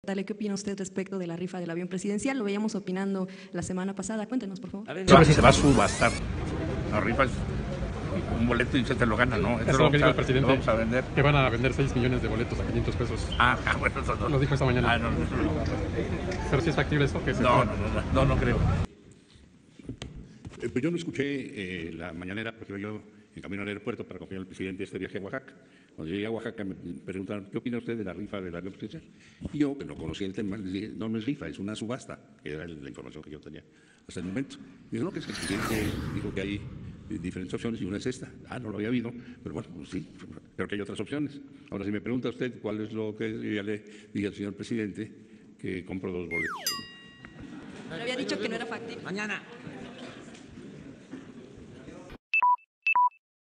Jiménez Espriú nos muestra 1% de capacidad y 99% de zalamería
El Secretario de @SCT_mx dijo que no era factible la rifa del AvionPresidencial ... hoy le corrigieron la plana y @JimenezEspriu comenta que compraría 2 boletos 🙄🦆